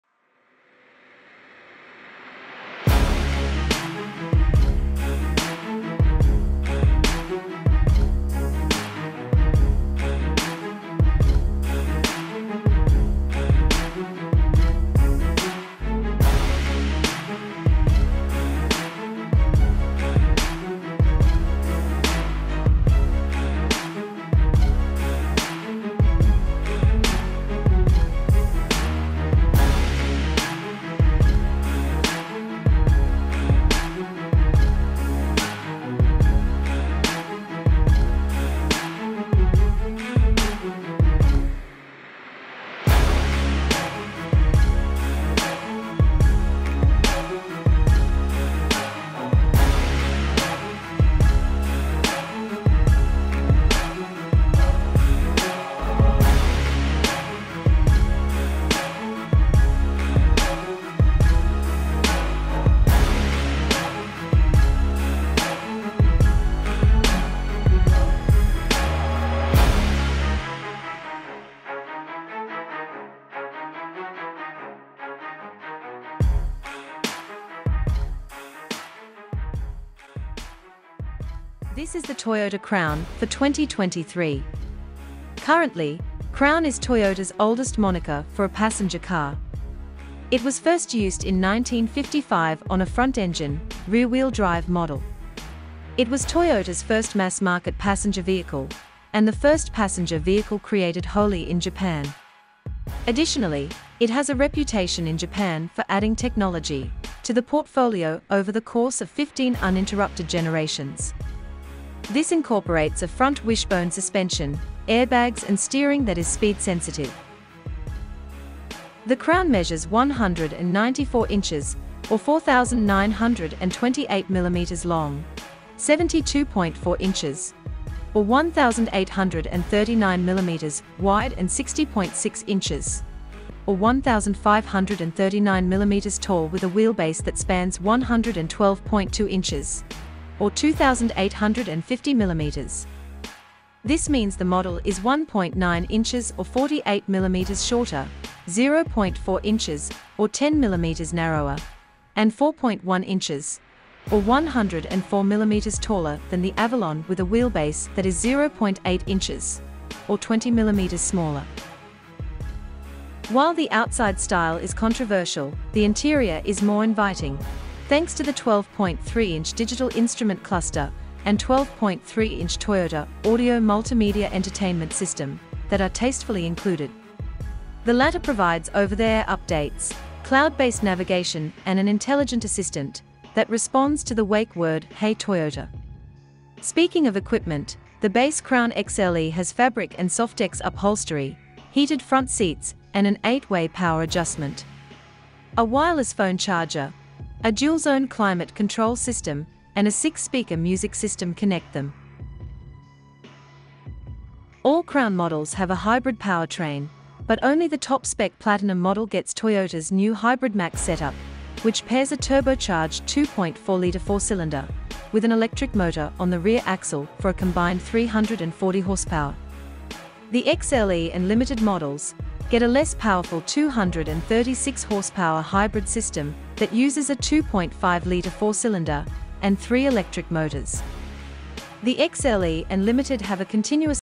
New 2023 Toyota CROWN - Stylish Car Preview Driving and Specs
The Toyota Crown was the first Japanese car to be sold in the United States, but sales were discontinued in 1972 after 14 years.

Despite its death in America, the Crown continued to live overseas and it turned into something rather interesting as the Japanese model featured rear-wheel drive and a powerful hybrid powertrain.

The car is now making its triumphant return to America, but it’s not the Crown many people were expecting. Instead, Toyota decided to reimagine the full-size sedan for the crossover era.

With the Avalon set to bow out at the end of the year, Toyota is effectively replacing it with a crossover-inspired Crown that features a “sophisticated higher ride-height design.” This approach has been tried by a handful of automakers, but models such as the Honda Accord Crosstour and Volvo S60 Cross Country weren’t terribly successful.

It remains unclear if the Crown will suffer a similar fate, but the model features a bZ4X inspired front fascia with slender headlights and a wide intake that is largely blocked off. Continuing further back, we can see plastic body cladding, a rakish windscreen, and 19- or 21-inch wheels.

The design will undoubtedly prove controversial and opinions will be further split by the Platinum’s available two-tone paint job. The latter sees black paint extend from the hood to the trunk, while being contrasted by an Oxygen White, Heavy Metal, Supersonic Red, or Bronze Age body.

Putting paint aside, the Crown measures 194 inches (4,928 mm) long, 72.4 inches (1,839 mm) wide, and 60.6 inches (1,539 mm) tall with a wheelbase that spans 112.2 inches (2,850 mm). This means the model is 1.9 inches (48 mm) shorter, 0.4 inches (10 mm) narrower, and 4.1 inches (104 mm) taller than the Avalon with a wheelbase that is 0.8 inches (20 mm) smaller.

While the exterior styling will prove divisive, the interior is more appealing as it comes nicely equipped with a 12.3-inch digital instrument cluster and a 12.3-inch Toyota Audio Multimedia infotainment system.  The latter offers cloud-based navigation, over-the-air updates, and an Intelligent Assistant that responds to the “Hey, Toyota” wake word.

Speaking of equipment, the entry-level Crown XLE features heated front seats with eight-way power adjustment as well as fabric and Softex upholstery. They’re joined by a dual-zone climate control system, a wireless smartphone charger, and a six-speaker audio system.

Under the hood, there’s a standard hybrid powertrain that features a 2.5-liter four-cylinder engine, two electric motors, an electronically controlled continuously variable transmission, and a newly developed high-output bipolar nickel-metal hydride battery. Toyota didn’t disclose performance specifications, but said the XLE and Limited variants are expected to return 38 mpg..